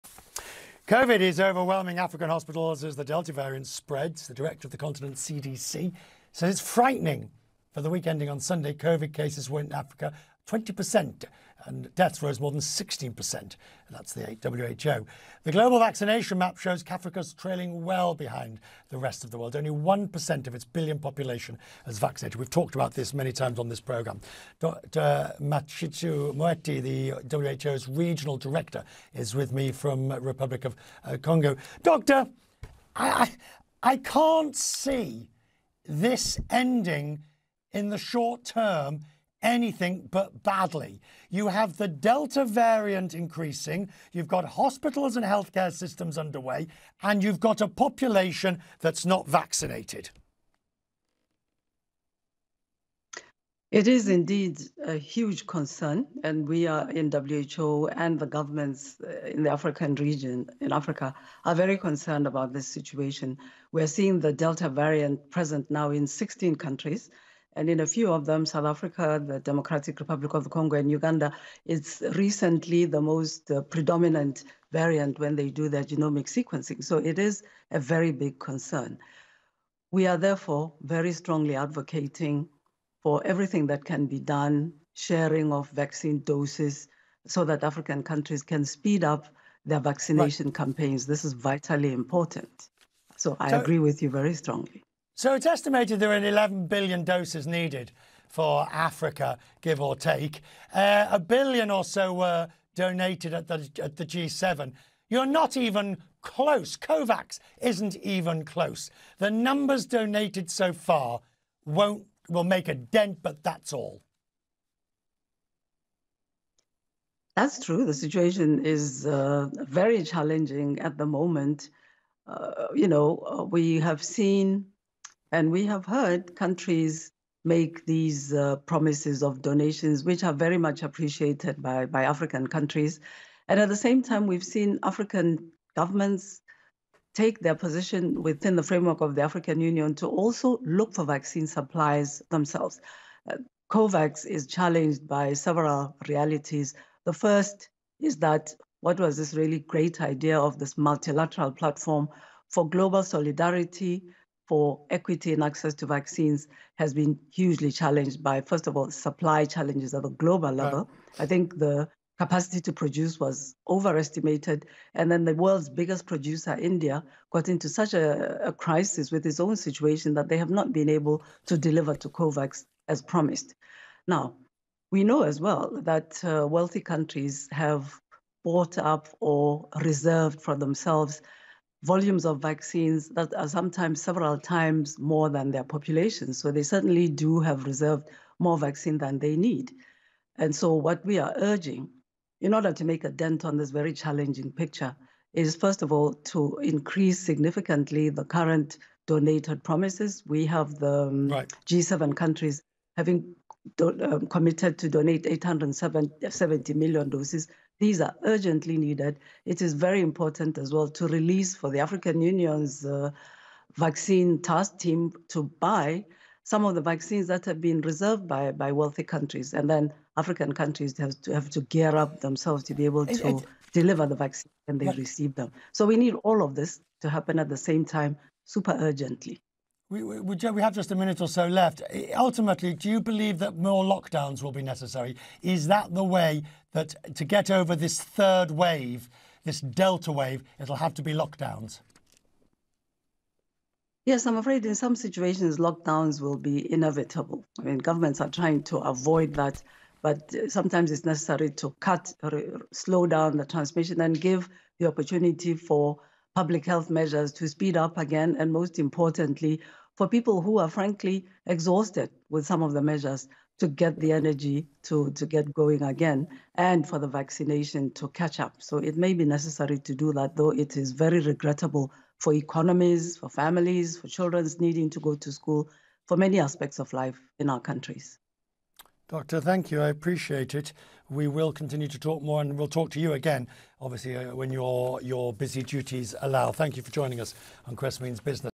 Delta variant spreads as Africa lags on vaccinations
With the Delta variant on the rise and only 1% of Africa's population vaccinated, there is cause for "huge concern," says W.H.O. Regional Director for Africa Dr. Matshidiso Moeti. "In some situations, lockdowns will be inevitable," although "governments are trying to avoid that."